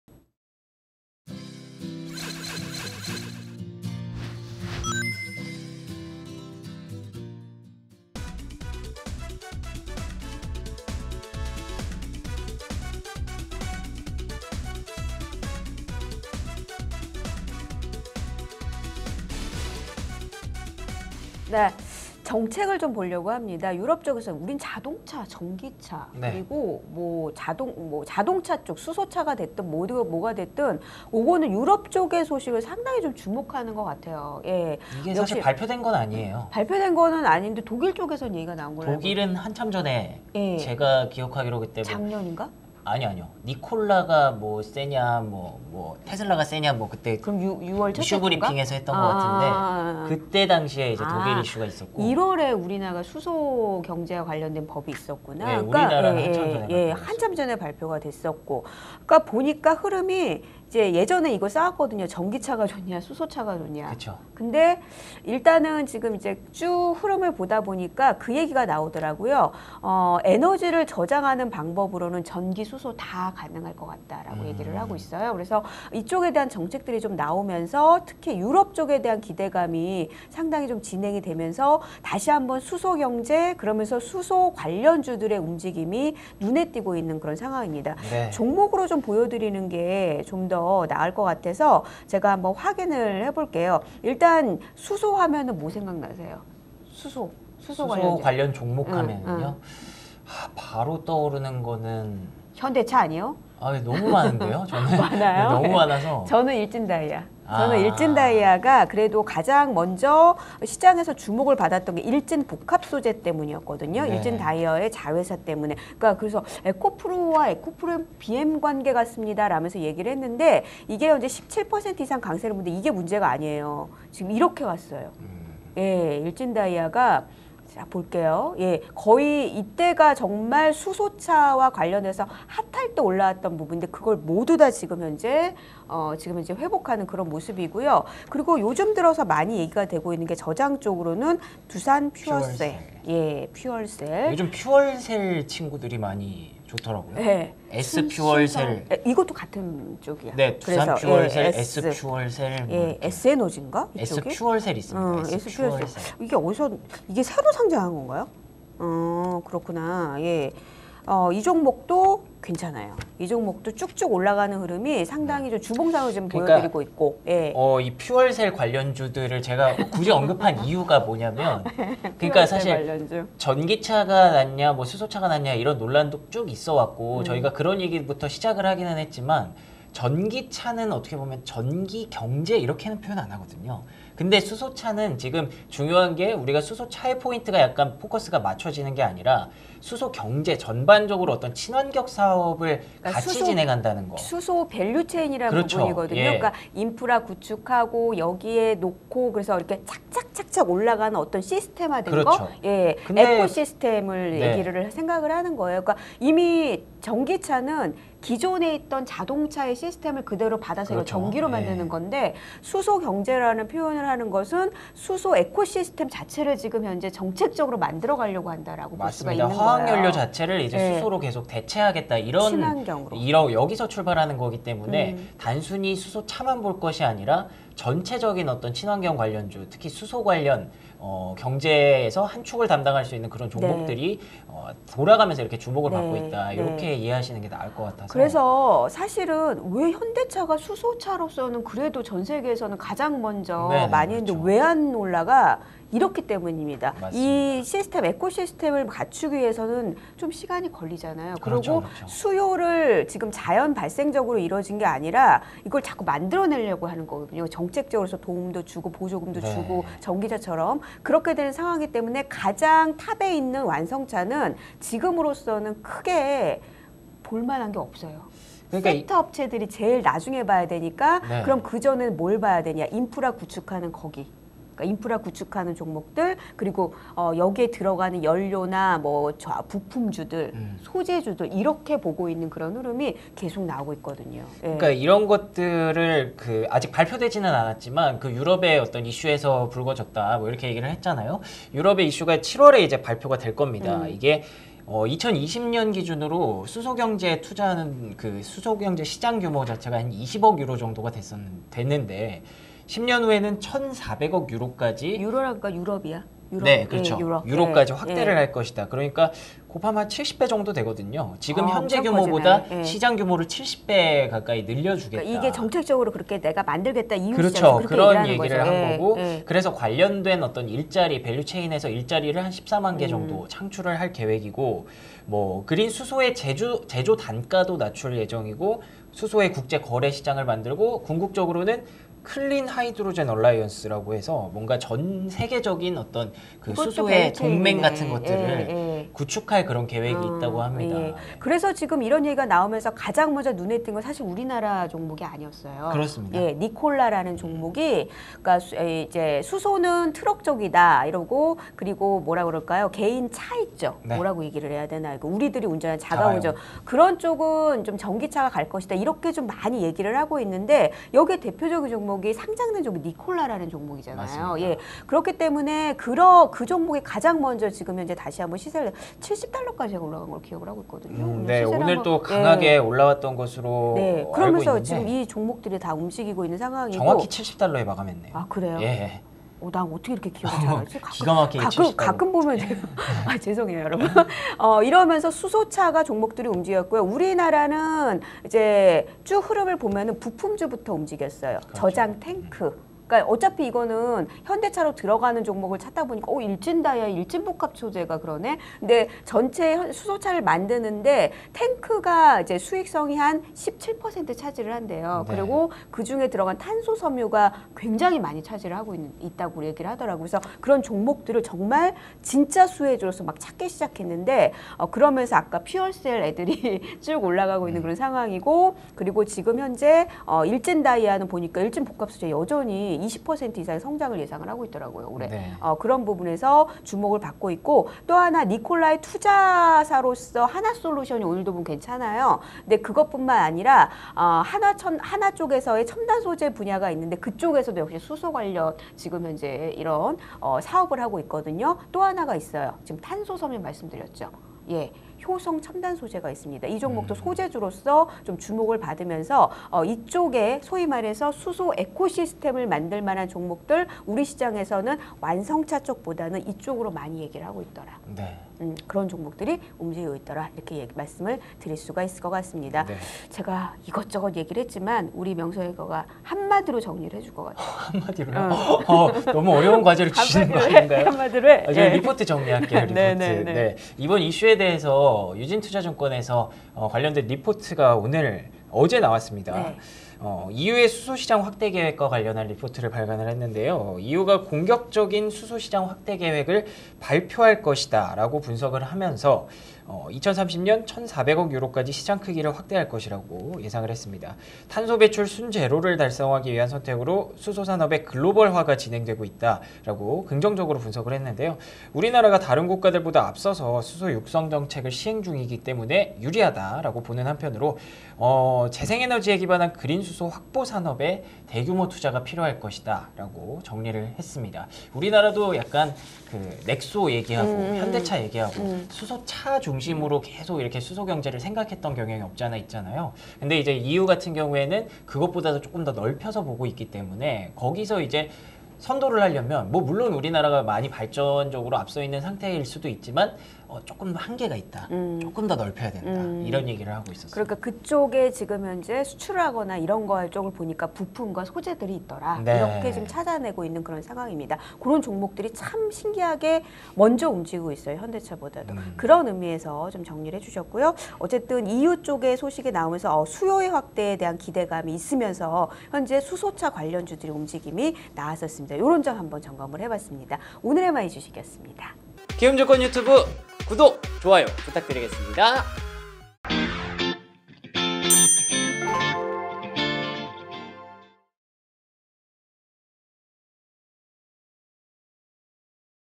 EU, 수소경제 구축계획 발표 기대... 관련주 전망은? (오늘의 마이주식.종목분석/20.06.23)
1. 수소경제 관련 정책으로 수소차 주가도 날개 날았나

2. 니콜라 중심으로 수소차 투자 지금도 괜찮을까

김순영, 박명석 채널K 앵커